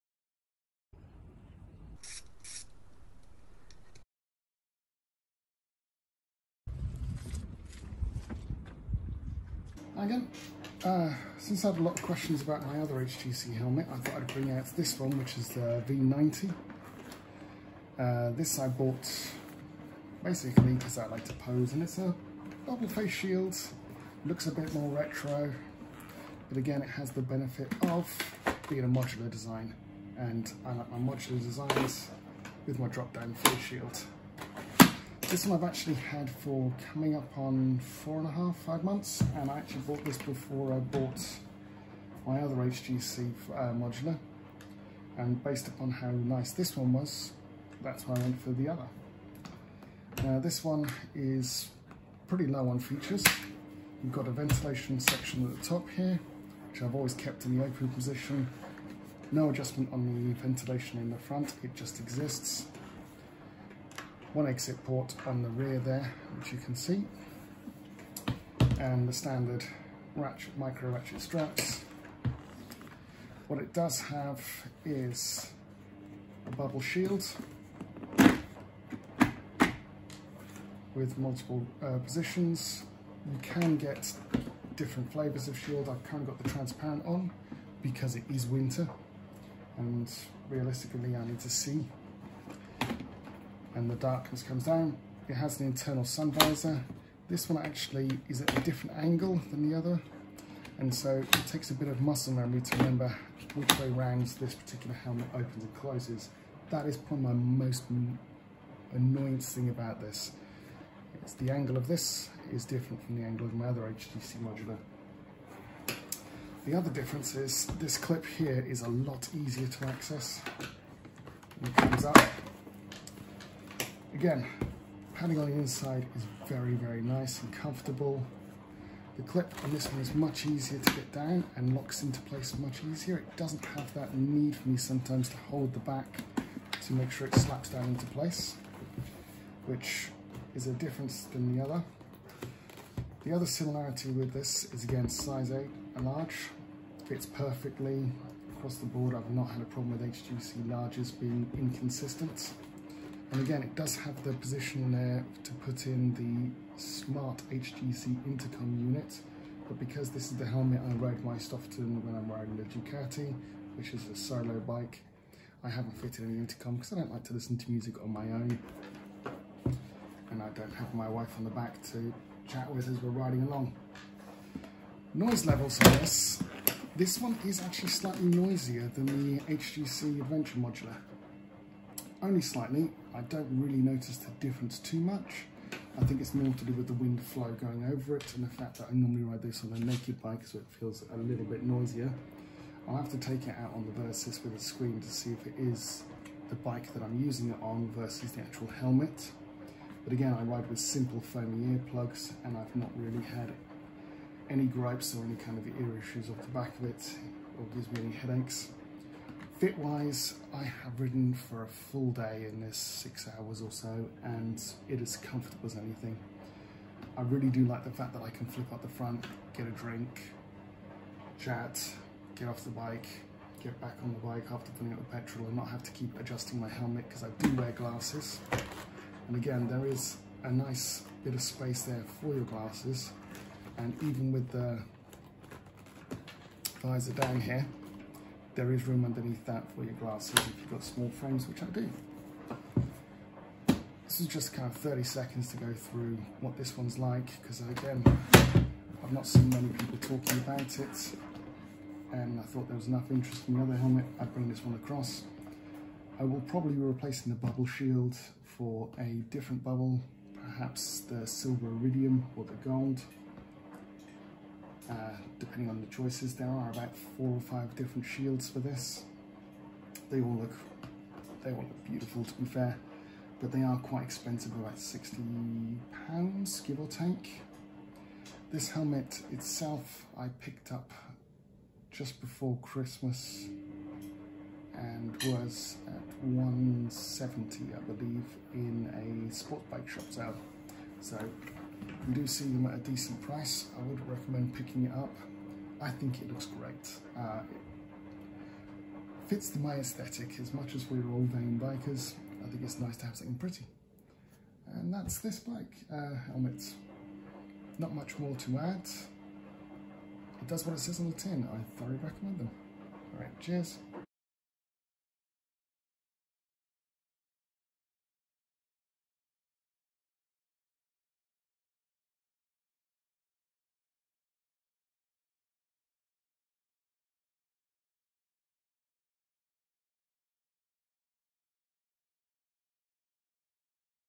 HJC V90 Owners Review
A review after three months of ownership of my HJC V90.This was purchased in the sportsbikeshop sale before Christmas as a bit of vanity for when I'm riding my Ducati.